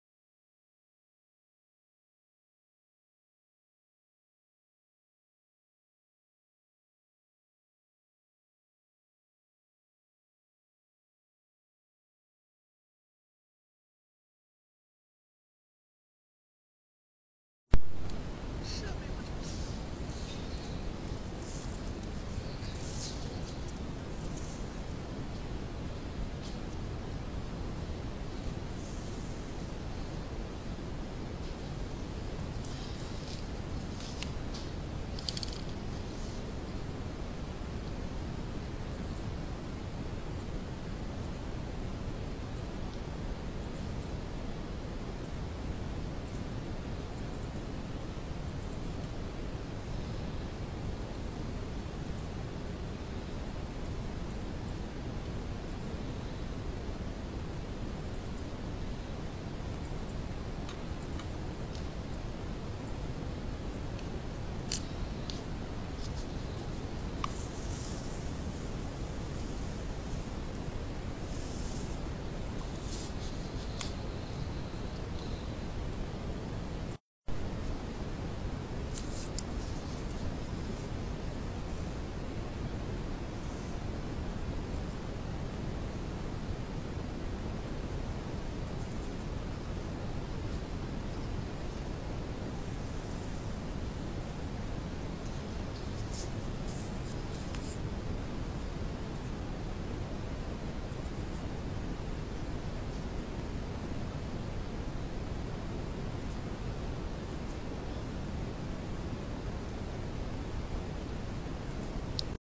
nanocoating纳罗可 纳米防水沙/盛水沙/憎水沙 细沙
纳罗可涂料自主研发生产。
纳米涂料和普通沙子混合、干燥后，沙子即可防水。可用于防水层、沙漠改造、沙漠球场建设等。